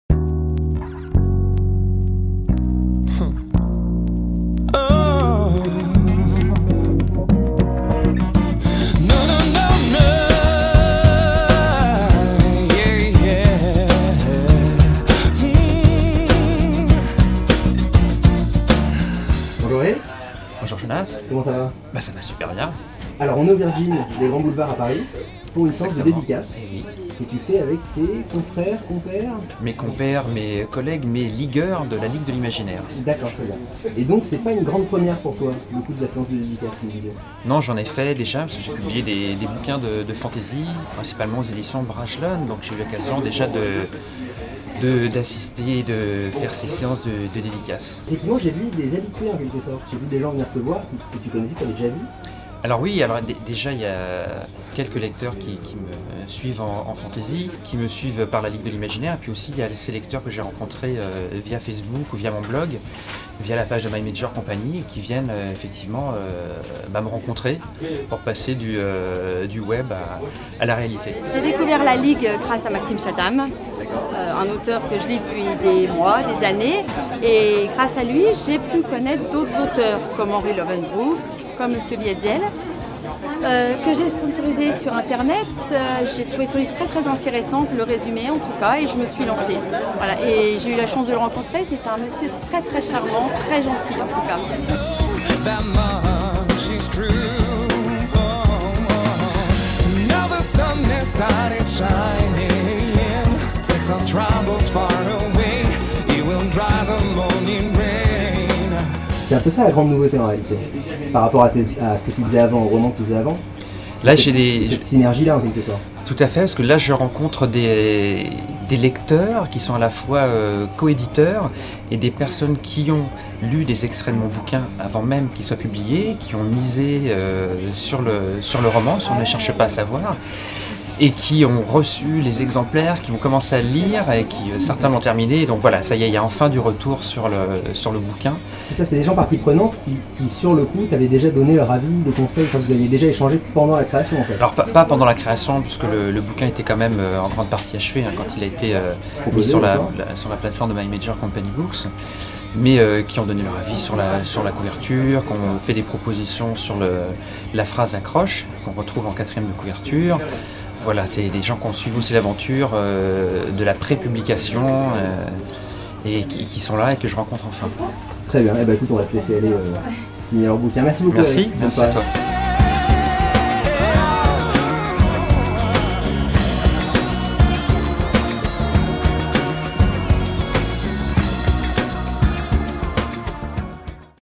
Erik Wietzel en dédicace
Revivez Erik Wietzel en séance de dédicace. Vous aussi devenez éditeur sur mymajorcompanybooks.com